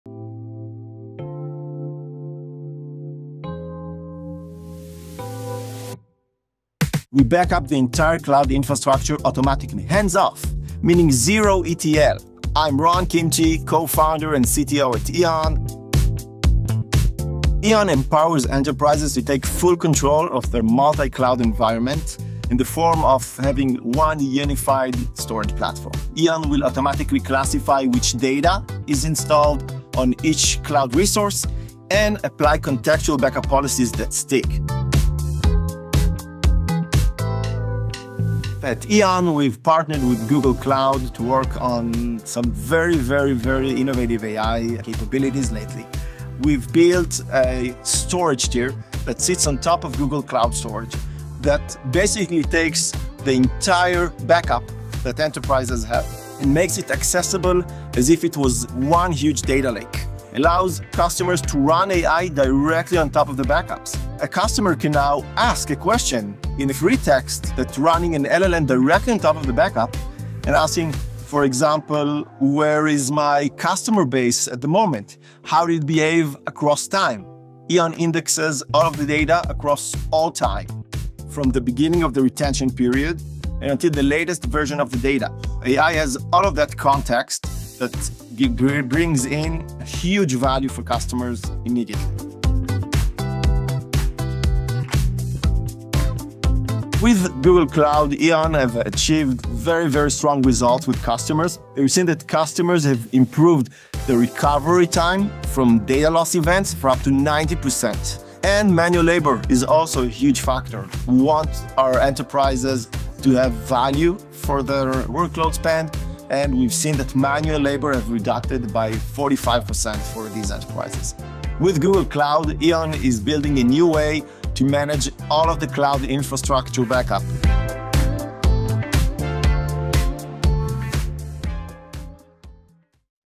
New Way Now: Eon transforms cloud backups to AI-ready data lakes
*Featured in this video:* Ron Kimchi, Co-founder and CTO at Eon

*Executive Summary:* Eon is a cloud-native, autonomous backup platform that unifies cloud backup posture management with a backup-optimized storage tier built on Google Cloud. It transforms cloud backups into instantly usable data lakes for AI, analytics, and recovery—eliminating silos and reducing operational and storage costs. Customers have achieved up to a 50% cost reduction in secondary storage used for backup. In some extreme cases, cost savings reached up to 98%. Enterprises have seen a 45% reduction in manual labor related to data protection and management.

*Challenge:* Enterprises frequently struggle with data loss, as demonstrated by a customer who faced a ransomware attack and discovered that a large percentage (roughly 40%) of their infrastructure was not properly protected or recoverable. This issue arises because centralized backup teams often lack awareness of new resources and data movements generated by distributed application teams using various cloud services, leading to misconfigurations and improper data retention. Additionally, traditional data analytics processes require extensive ETL (extract, transform, load) from numerous disparate databases, creating pervasive data silos that hinder efficient data access and analysis for AI purposes.

*Solution:* Eon transforms complex, multi-cloud backups into a live, queryable data lake—no restores, no ETL—enabling instant access for analytics, AI, and granular recovery. This enables customers to run analytics and AI tools, such as BigQuery, directly on their backups without needing to restore any data or perform manual ETL processes. Users can search across backups, run SQL queries, and pluck individual files from any point in time. Furthermore, Eon allows customers to leverage large language models (LLMs) to ask free-text questions directly on the indexed backup data, gaining immediate context and value from historical data across the entire retention period.

*Result:* Eon's collaboration with Google Cloud has yielded strong, measurable results for customers. Enterprises have significantly optimized costs, achieving up to a 50% reduction in secondary storage for backups—with some extreme cases reaching savings as high as 98%. Furthermore, the partnership has drastically improved recovery time from data loss events by up to 90%, while also cutting manual labor associated with data protection and management by 45%.

*Interview highlights and key takeaways from Ron Kimchi, Co-founder and CTO at Eon:*

→ "Eon empowers enterprises to take full control of their multi-cloud environment in the form of having one unified, storage platform."

→ "We've built a storage tier that sits on top of Google Cloud Storage that basically takes the entire backup that enterprises have and makes it accessible, as if it was one huge data lake."

→ "Customers can run BigQuery directly on top of their backups without restoring any data. In addition, they can use RAG and agentic AI directly on top of the backup."

→ "Together with Google Cloud,  Eon has achieved very strong results for customers. We've seen that customers have improved the recovery time from data loss events up to 90%. In addition, we want our enterprises to have value for their workloads span, and we've seen that manual labor has been reduced by 45% for these enterprises."

*Google Cloud products used:* Google Cloud Storage, BigQuery